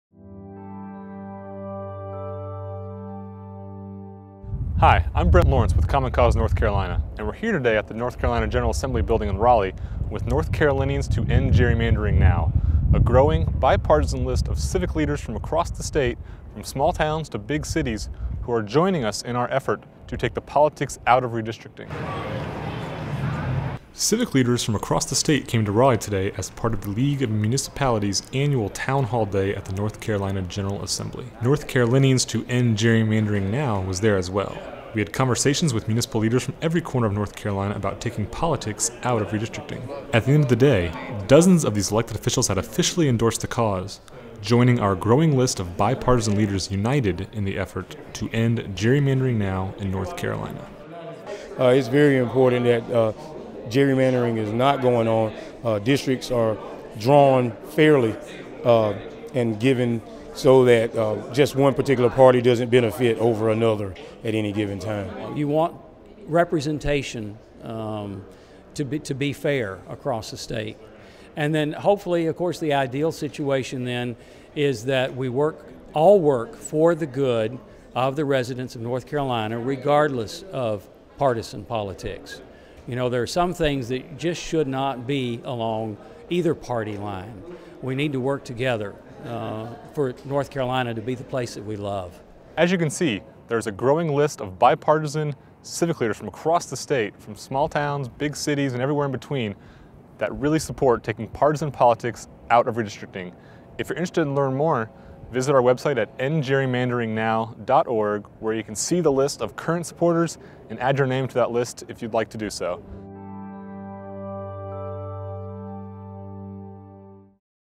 NC civic leaders voice support for redistricting reform
A growing number of local elected officials from across the state are calling for an end to gerrymandering in North Carolina.